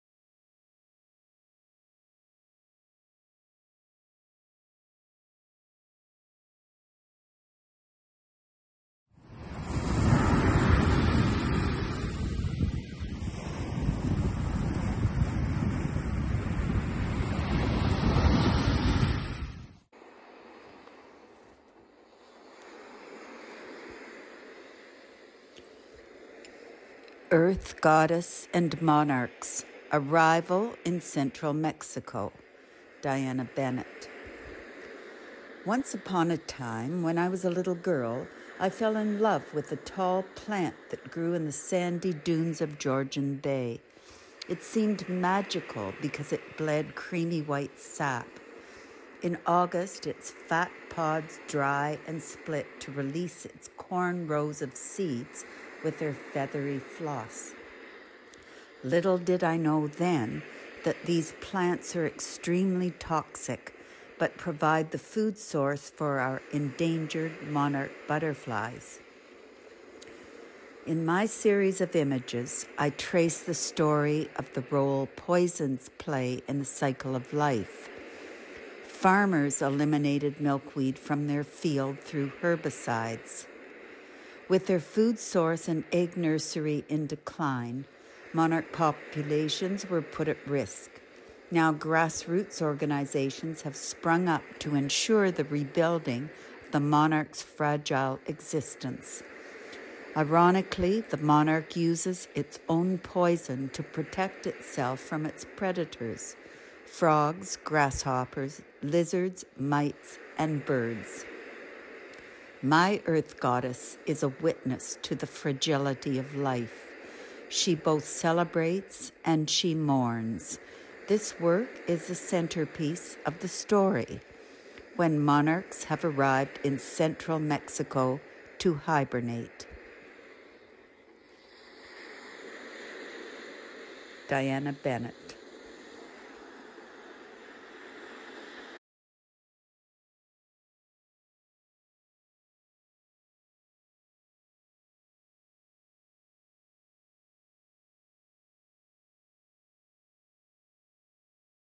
Diana Bennett
LETTERS TO THE EARTH: BETWEEN DESPAIR AND HOPE. The initiative is a conversation about the environmental crisis happening around the world. To preserve the future for our children and future generations, the time to act is now. Artist, Diana Bennett speaks through her art to draw attention to gradual extinction of Monarch Butterflies.